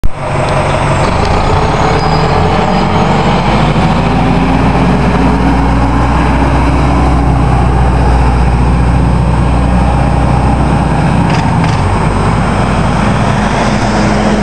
"Must Wait For The Light...or not!"
NovaBus RTS-06 #4905 was on the Q84 traveling along Archer Av in Jamaica and was heading towards 238th St and 120th Av in Laurelton, Queens.

If you can see it, notice the traffic lights are red when the bus takes off. Talk about impatient, huh? LOL! I guess this must either be his last trip OR the driver was running late. Nonetheless, that bus had a great sound on take-off though. Enjoy!